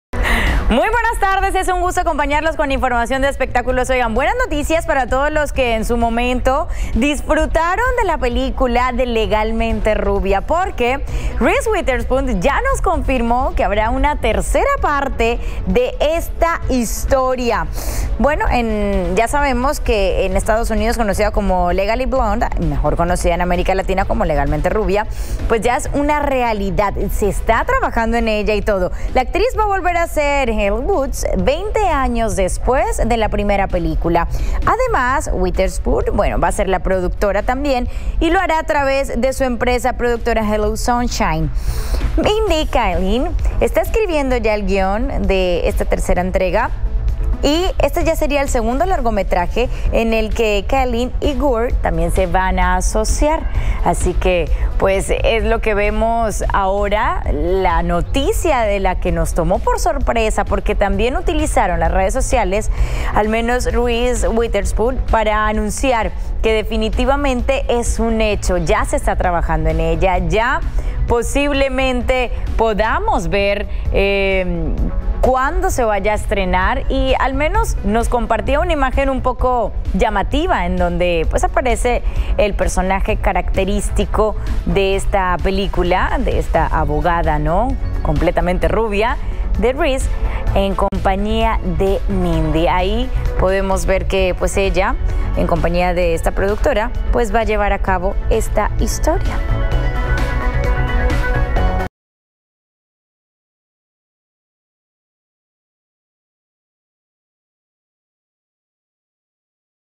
Reese Witherspoon confirma "Legalmente Rubia 3"
La actriz Reese Witherspoon confirmó que la tercera parte de Legally Blonde, conocida en Latinoamérica como Legalmente Rubia.